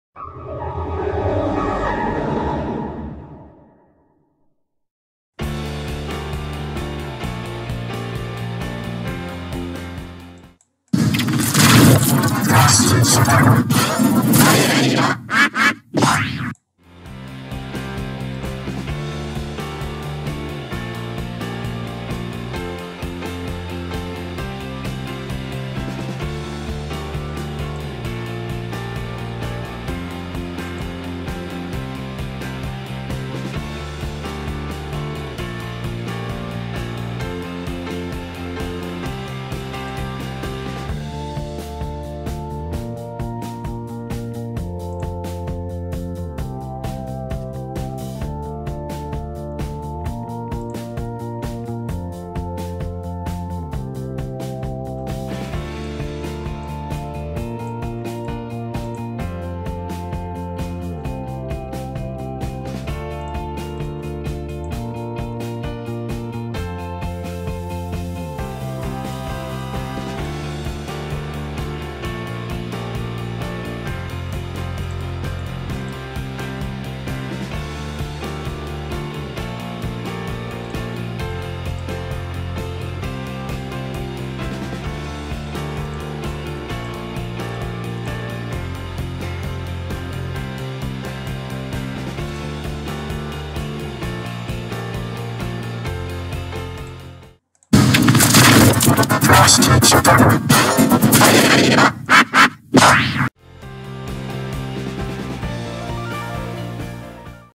How To Make The Original G-Major 72 On Vegas Pro
Made today on Wednesday, ‎August ‎19, ‎2020,‏ ‏‎2:19:14 pm to 2:19:54 pm

Yes, this is the original G-Major 72 made on YouTube video editor on November 15, 2016.

Effect by Klasky123 The Editor

Klasky Csupo in G Major 72 (Made By YouTube Editor)